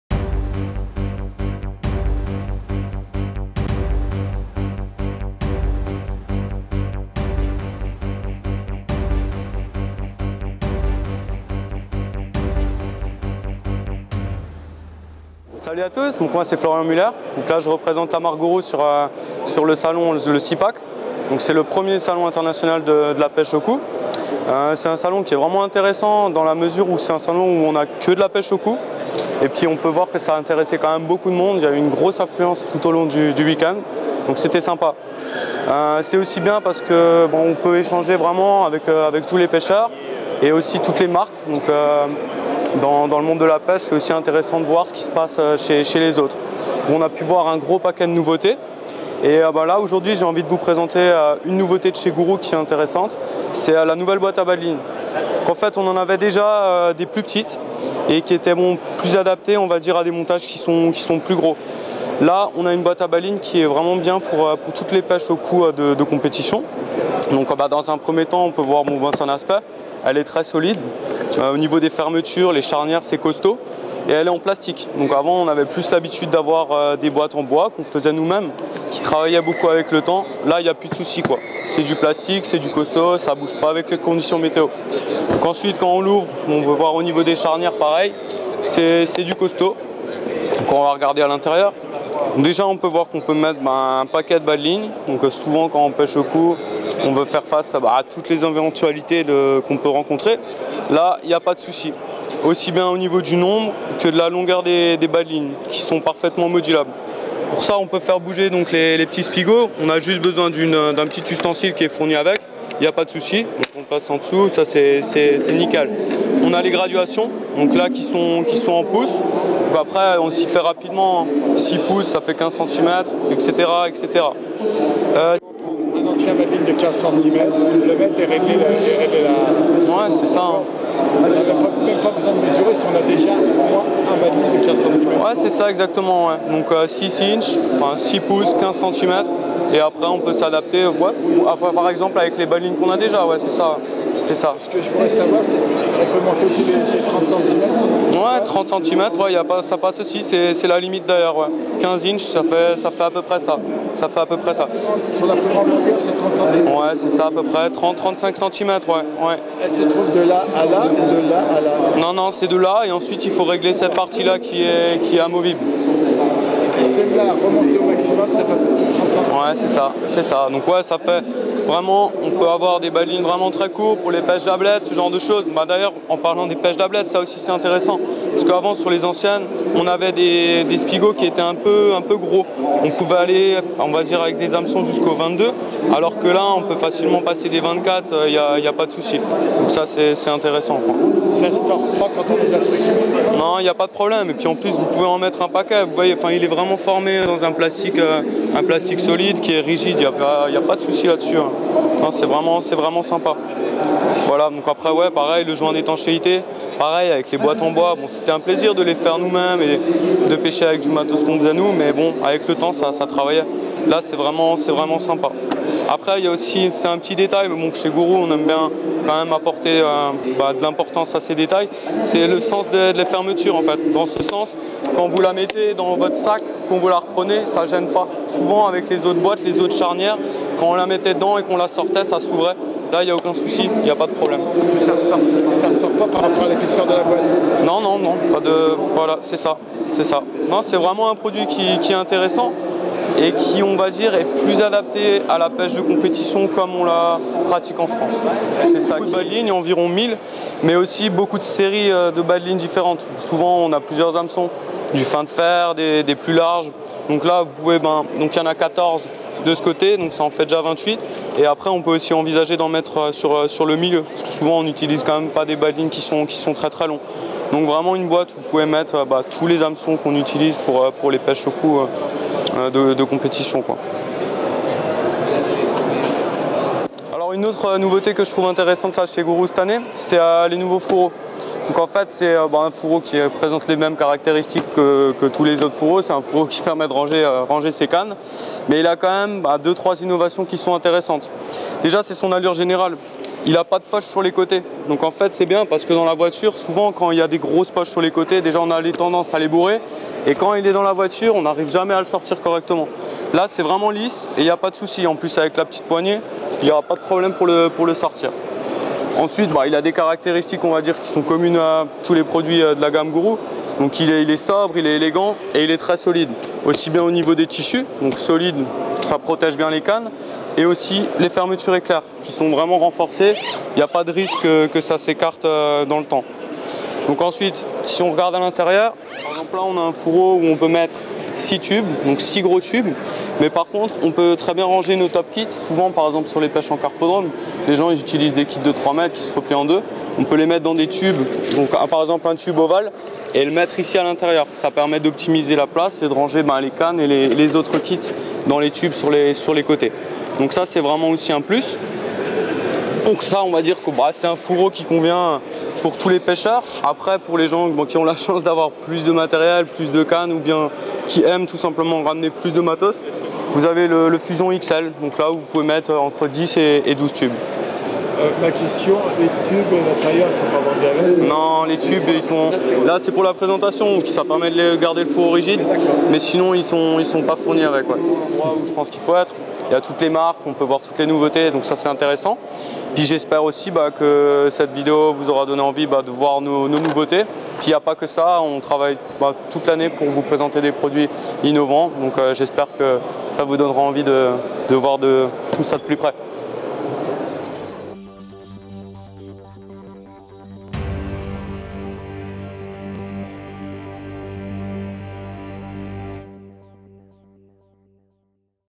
SALON SIPAC GURU
Bonjour  à tous et toutes,

Voici les vidéos du salon du Sipac  qui s’est déroulé dans la ville d’Amiens le 26, 27,28, octobre 2018.

 Pour continuer cette nouvelle  Série de vidéos :

Voici la vidéo que j’ai réalisée avec Florian Muller  de la marque Guru

Il nous présente:

 La nouvelle boite à hameçons XL
.Les fourreaux